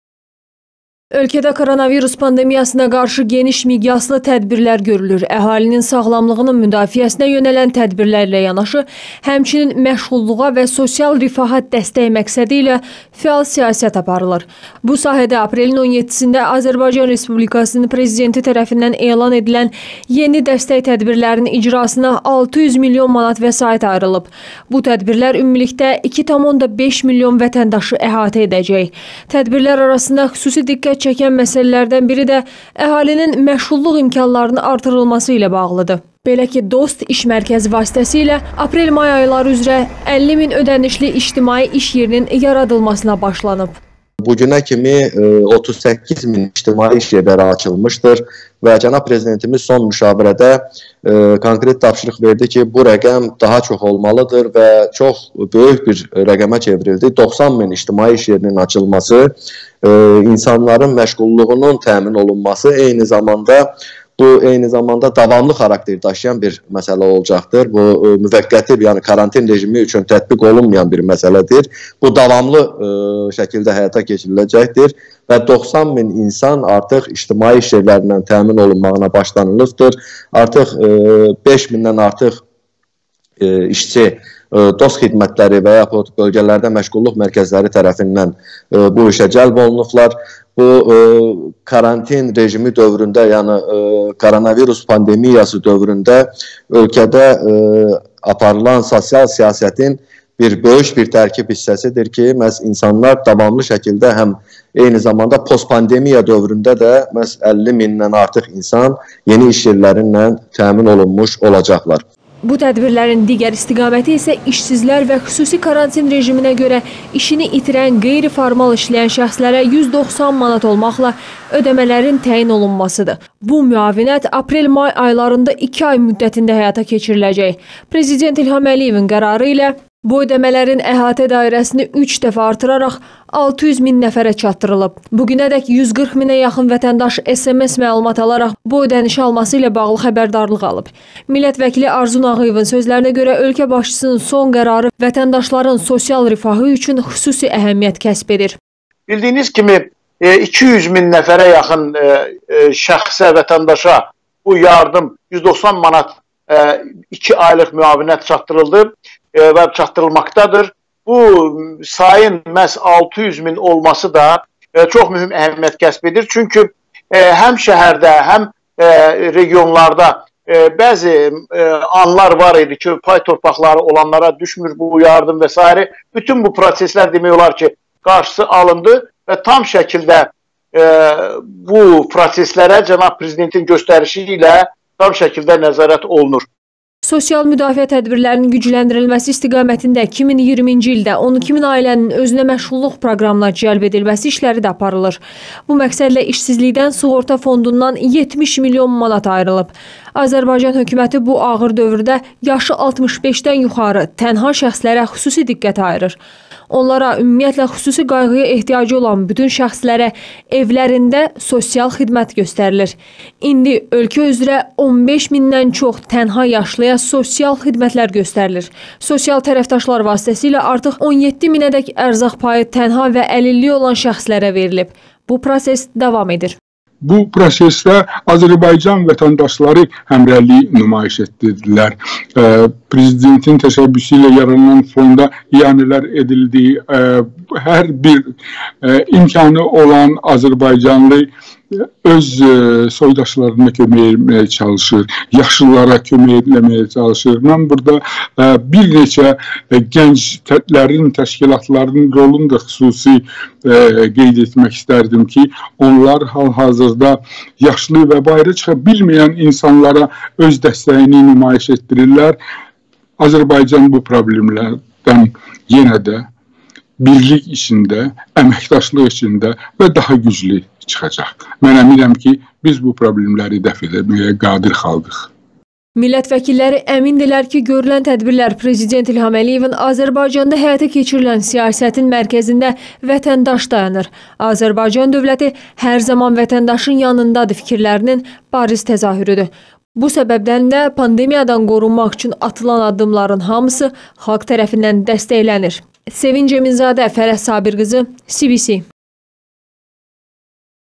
YENİ DƏSTƏK TƏDBİRLƏRİNƏ 600 MİLYON MANAT AYRILIB
Ölkədə koronavirus (COVID-19) pandemiyasına qarşı genişmiqyaslı tədbirlər görülür. Əhalinin sağlamlığının müdafisəinə yönələn tədirlərlə yanaşı, həmçinin məşğulluğa və sosial rifaha dəstək məqsədilə fəal siyasət aparılır. Bu sahədə aprelin 17-də Azərbaycan Respublikasının Prezidenti tərəfindən elan edilən yeni dəstək tədbirlərinin icrasına 600 milyon manat vəsait ayrılıb. Bu tədbirlər ümumilikdə 2,5 milyon vətəndaşı əhatə edəcək.
Tədbirlər arasında xüsusi diqqət çəkən məsələlərdən biri də əhalinin məşğulluq imkanlarının artırılması ilə bağlıdır. Belə ki, DOST İş Mərkəzi vasitəsi ilə aprel-may ayları üzrə 50 min ödənişli ictimai iş yerinin yaradılmasına başlanıb. Millət vəkili Cavid Osmanov bu barədə danışıb.
Bu tədbirlərin digər istiqaməti isə işsizlər və xüsusi karantin rejiminə görə işini itirən qeyri-formal işləyən şəxslərə 190 manat olmaqla ödəmələrin təyin olunmasıdır. Bu müavinət aprel-may aylarında 2 ay müddətində həyata keçiriləcək. Prezident İlham Əliyevin qərarı ilə bu ödəmələrin əhatə dairəsini 3 dəfə artıraraq 600 min nəfərə çatdırılıb. Bugünədək 140 minə yaxın vətəndaş SMS məlumat alaraq bu ödənişi alması ilə bağlı xəbərdarlıq alıb. Millət vəkili Arzu Nağıyevin sözlərinə görə ölkə başçısının son qərarı vətəndaşların soaisl rifahı üçün xüsusi əhəmiyyət kəsb edir.
Azərbaycan hökuməti bu ağır dövrdə yaşı 65-dən yuxarı tənha şəxslərə xüsusidiqqə ayırır. İndi ölkə üzrə15 mindən çox  tənha yaşlı insane sosial xidmətlər göstərilir. Sosial tərəfdaşlar vasitəsilə artıq 17 minədək ərzaq payı tənha və əlilliyi olan şəxslərə verilib. Bu proses davam edir.
Millət vəkilləri əmindirlər ki, görülən tədbirlər Prezident İlham Əliyevin “Azərbaycanda həyata keçirilən siyasətin mərkəzində vətəndaş dayanır”, “Azərbaycan dövləti hər zaman vətəndaşın yanındadır” fikrilərinin bariz təzahürüdür.